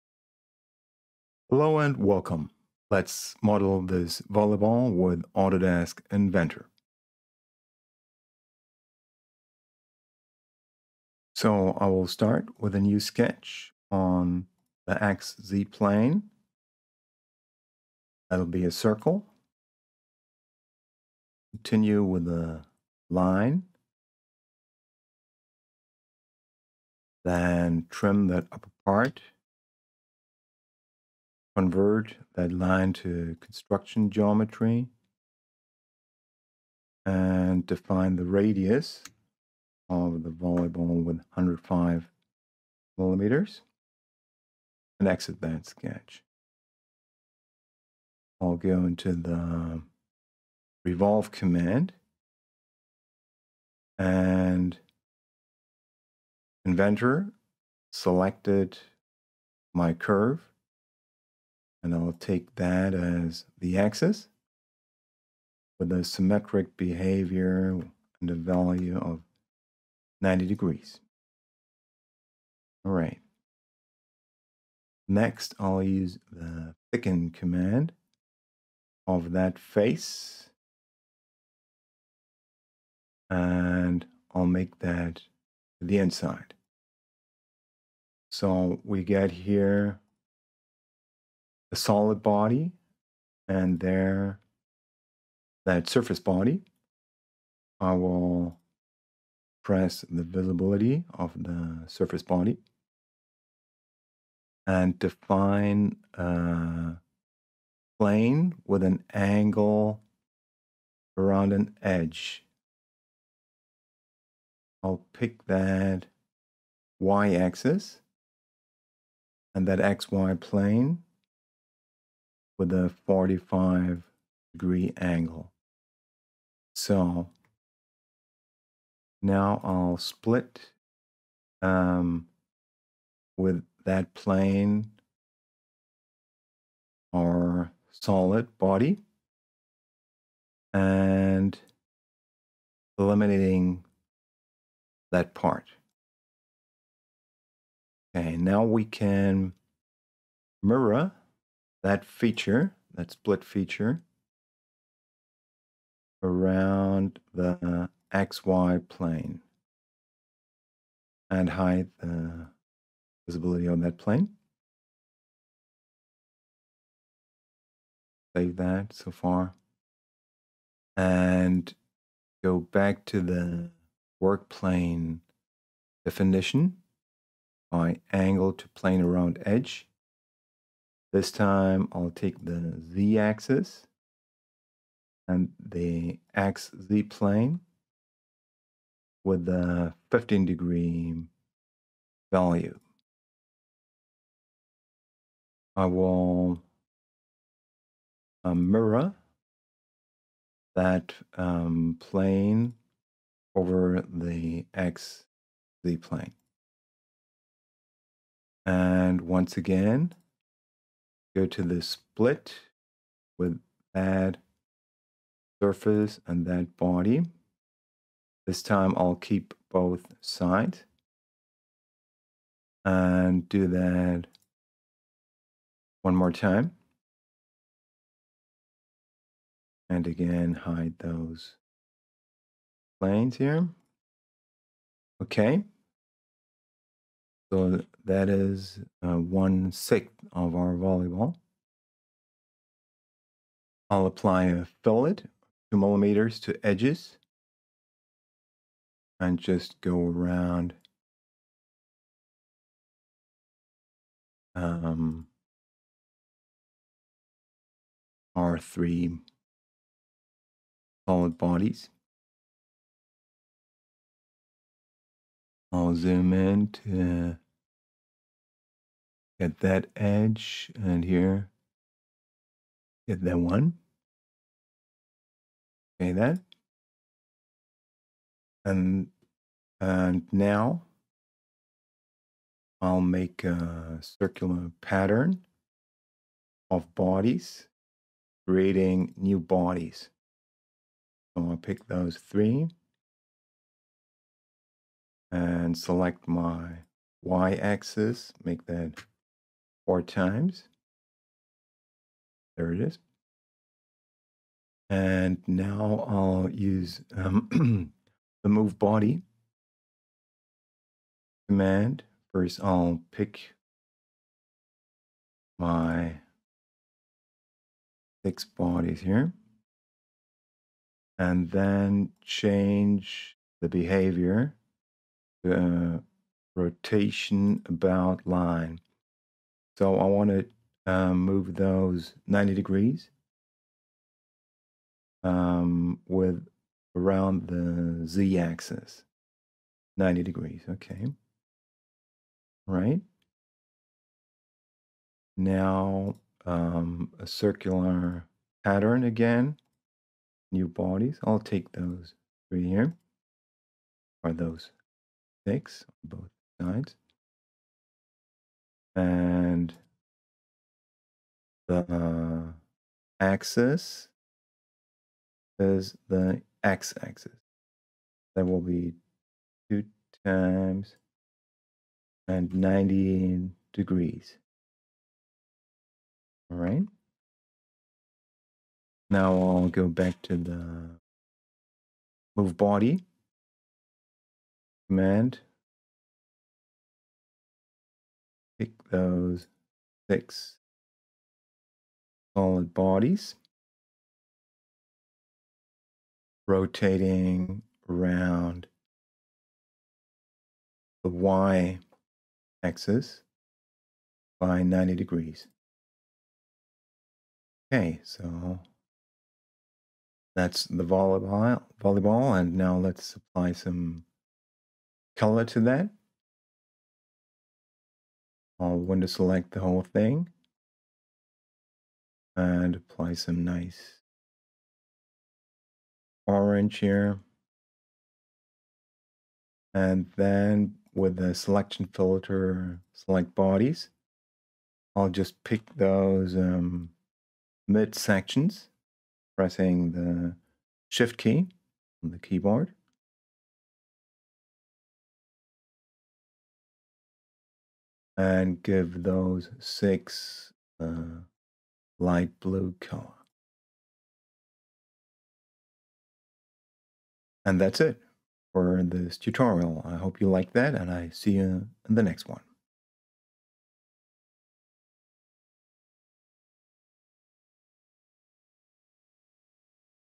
Step-by-Step CAD Tutorial for Modeling a Volleyball in Autodesk Inventor
Learn how to create a realistic volleyball model in Autodesk Inventor. This step-by-step tutorial will guide you through the process, covering essential techniques like surface revolve, plane creation, split body, circular pattern, and move body. Master these skills and elevate your 3D modeling abilities. Perfect for beginners and intermediate users alike, this tutorial will help you unleash your creativity and design your own unique volleyball.